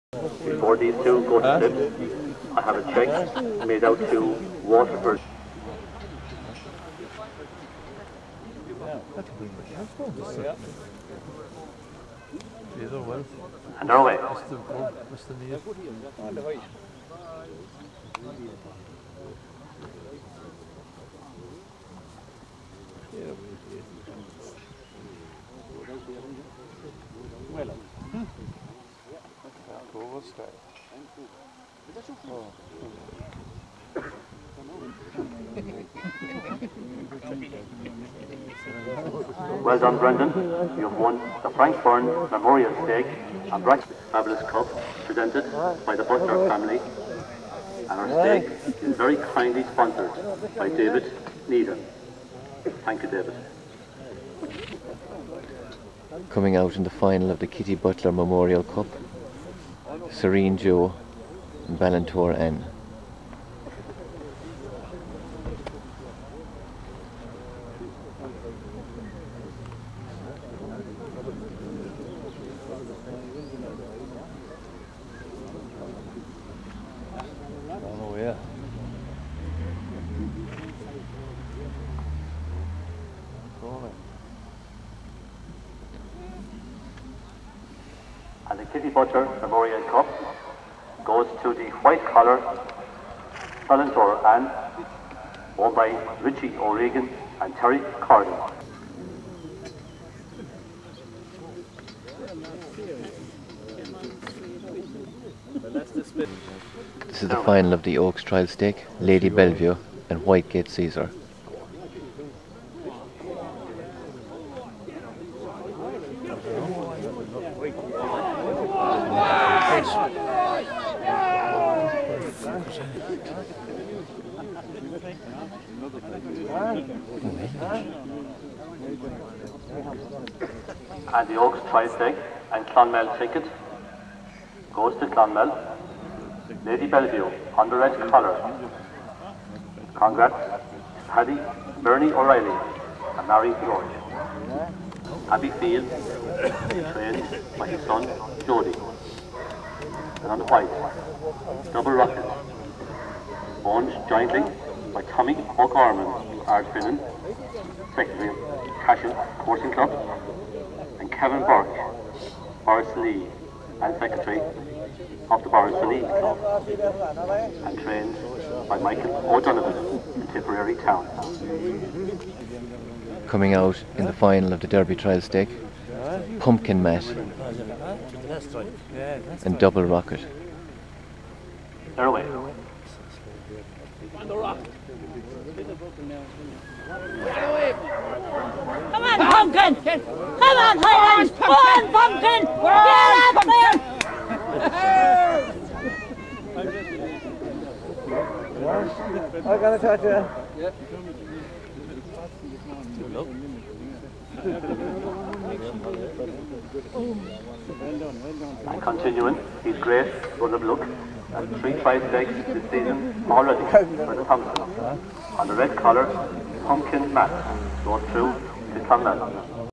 Clonmel & Kilsheelan 2007 -The Finals
Finals of Working Members , Kitty Butler ,Oaks and Derby trial stakes at Clonmel & Kilsheelan coursing meeting. (November 2007)